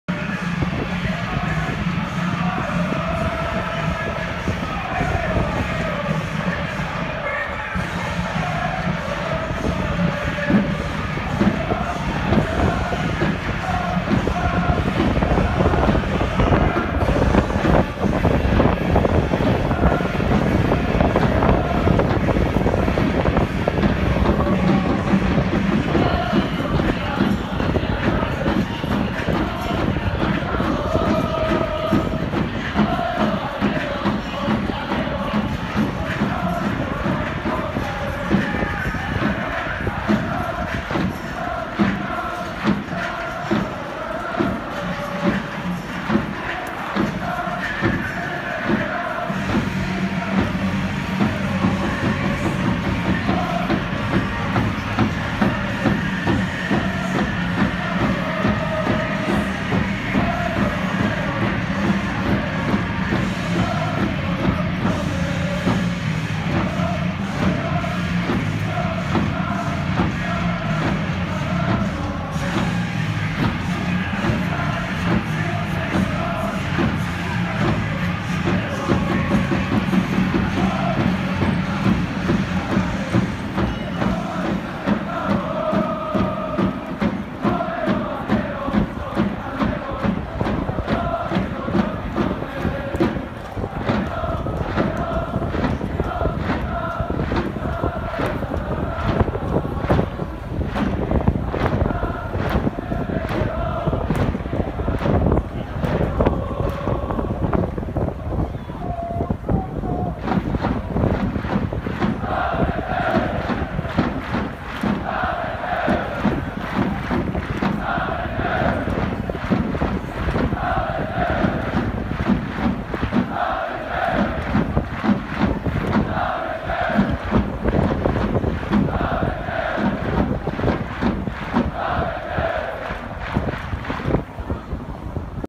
170225 新チャント(アルビレックス新潟戦)
17年2月25日Jリーグ第１節。サンフレッチェ広島VSアルビレックス新潟戦。サンフレッチェ広島・新チャント。
廣永遼太郎、塩谷司、千葉和彦、水本裕貴、青山敏弘、稲墻祥、ミキッチ、柏好文、森島司、フェリペ・シウバ、工藤壮人、中林洋次、野上結貴、高橋壮也、茶島雄介、清水航平、丸谷拓也、皆川佑介